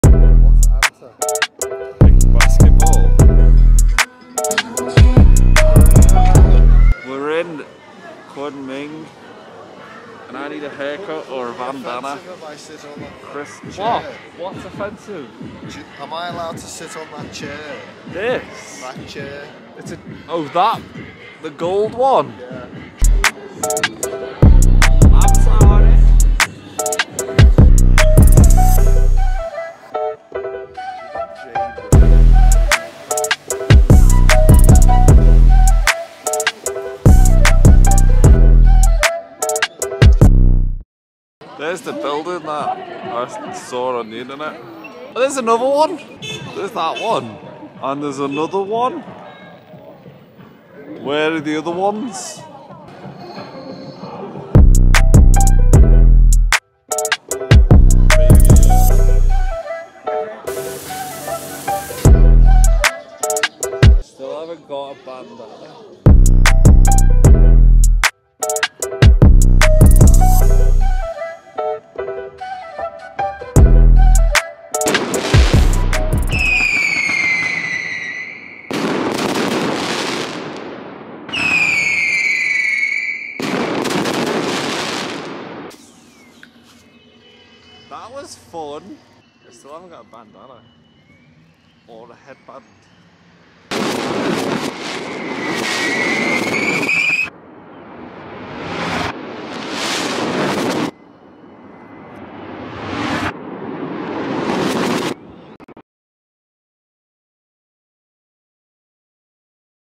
We arrived in Kunming
It's Saturday and we have arrived in Kunming.